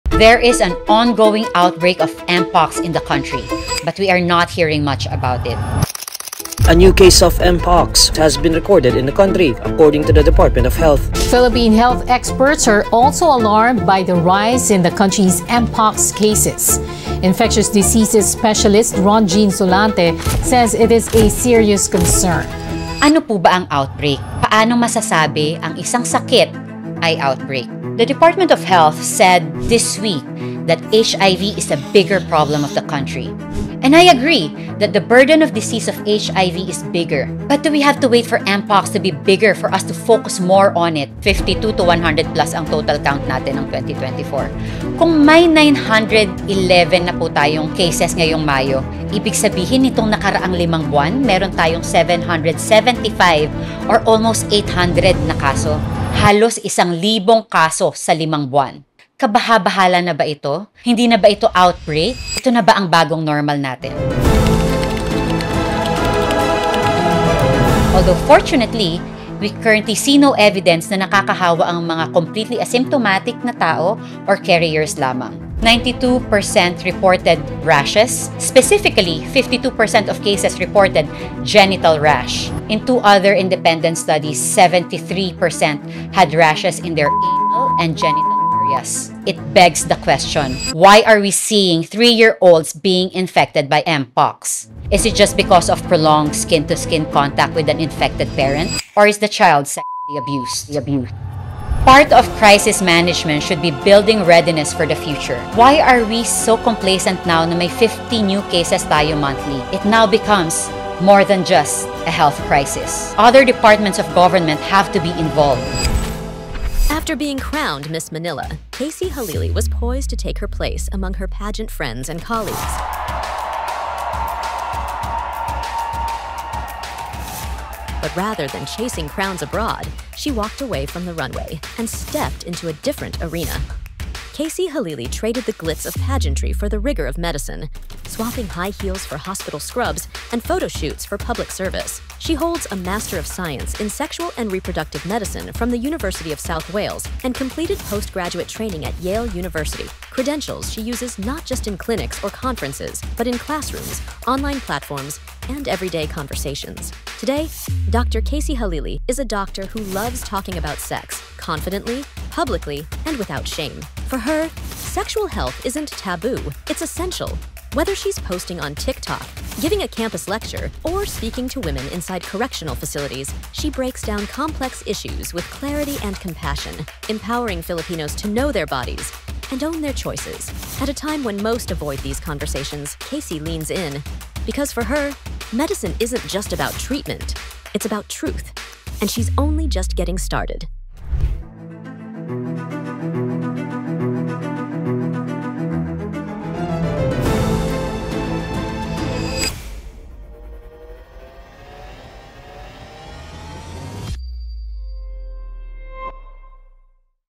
SHOCKING: There are now at least 900 confirmed cases of MPox in the Philippines
The Department of Health says there's no need to worry—but recent data and missing updates suggest otherwise. In this episode, Dr. KC Halili unpacks everything you need to know about the Mpox situation in the Philippines: the latest confirmed cases, how it spreads, why children getting infected raises red flags, and what the global trends are telling us.

Episode drops Thursday at 7 PM on YouTube, Facebook, TikTok, Instagram and X.